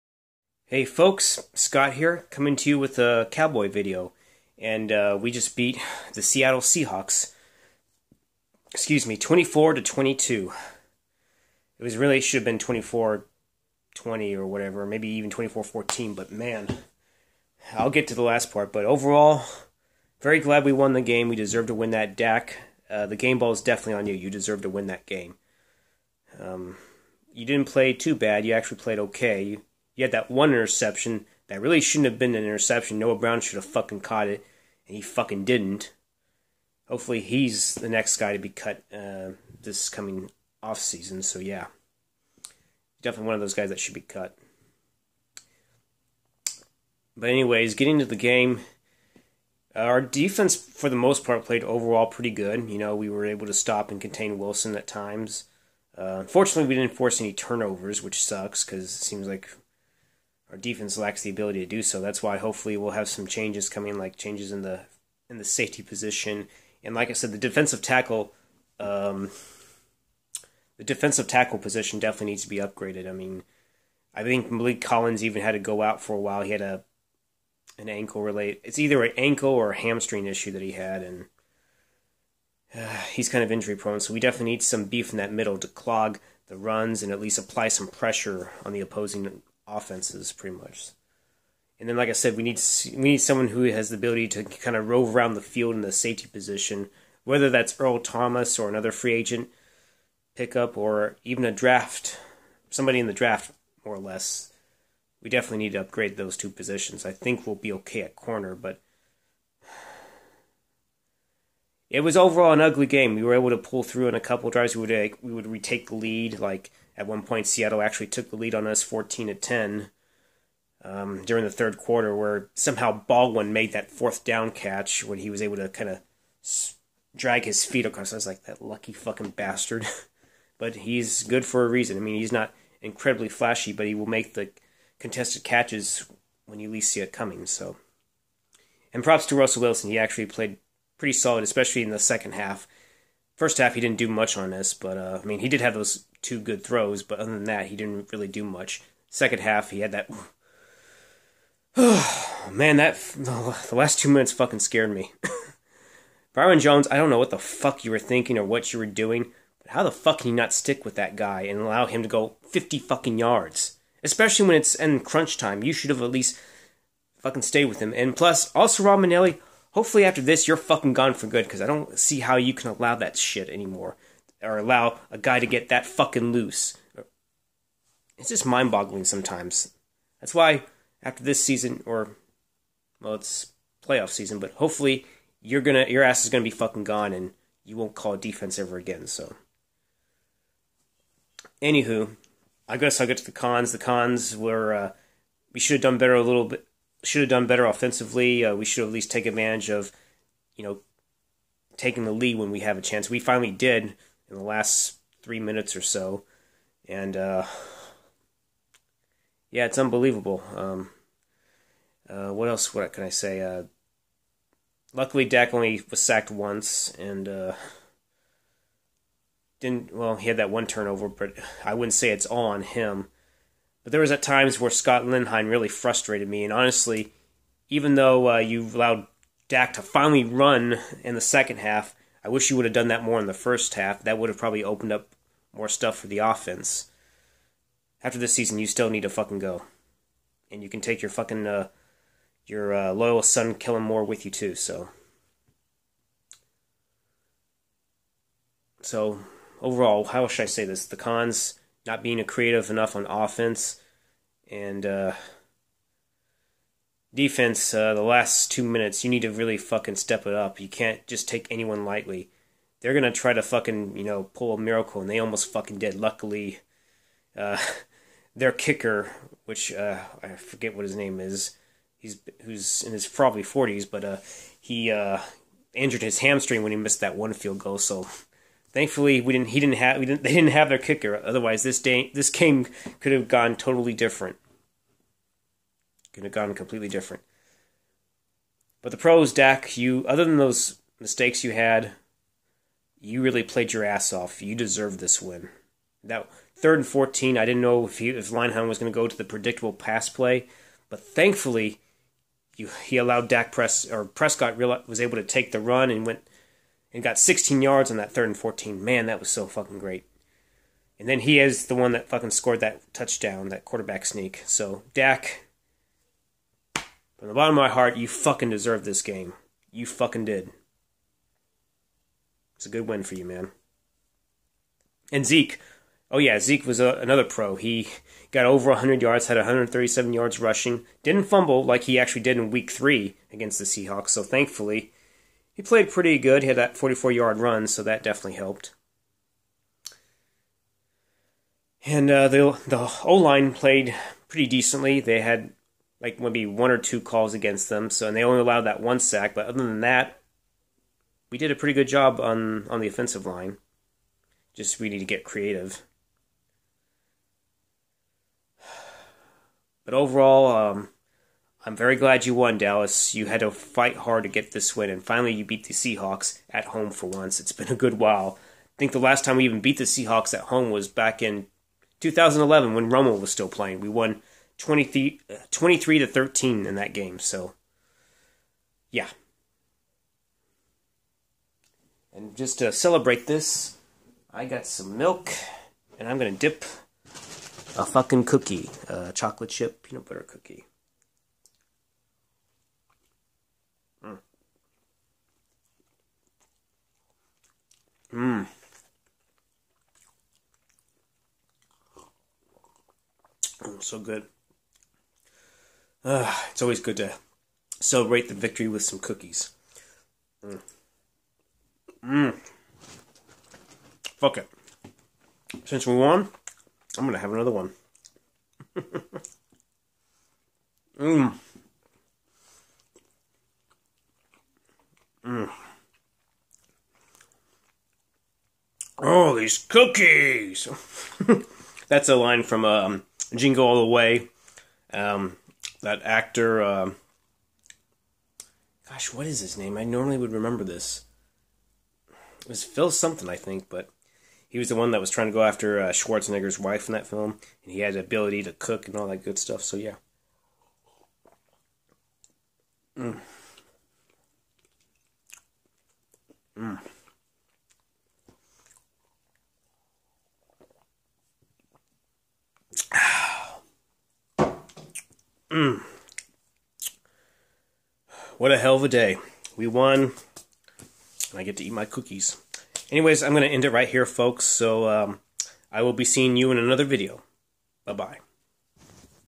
COWBOYS WIN, 24-22! NOT ONE AND DONE!
The Cowboys defeat the Seattle Seahawks 24-22 and advance to the divisional round.